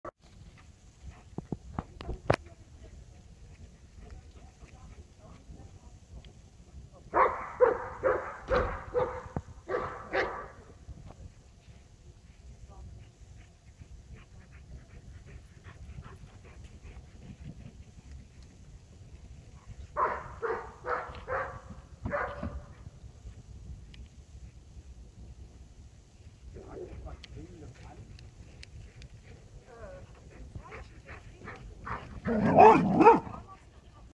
Nick, Kito & Romy - 04. November 2014
Happy Labrador Romy & ihr Hundeleben
Nick - Schäferhund, Kito - Schäferhundmix